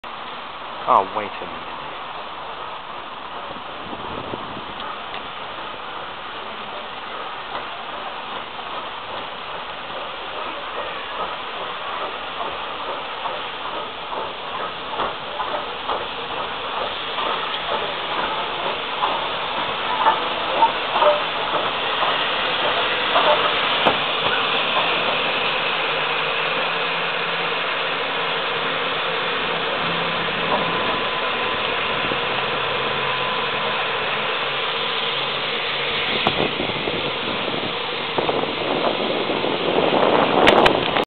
LNWR Super D 49395 arrives at Bury Bolton Street
Filmed at the East Lancs Railway Autumn steam gala